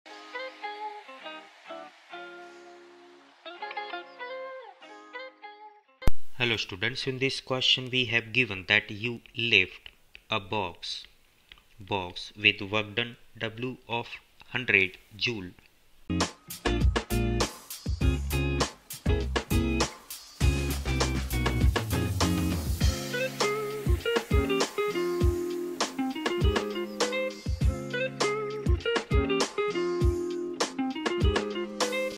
If you lift a box using 100 J of work within 5 s, how much power has been done? How much is the pow…
If you lift a box using 100 J of work within 5 s, how much power has been done? How much is the power if it x27;s lifted using 10 J of work?